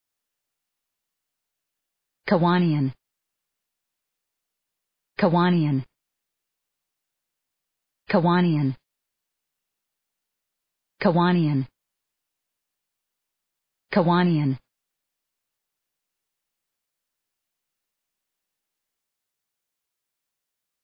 How to pronounce Kiwanian
Kiwanian American English pronunciation. How to pronounce Kiwanian correctly. How to say Kiwanian in proper American English.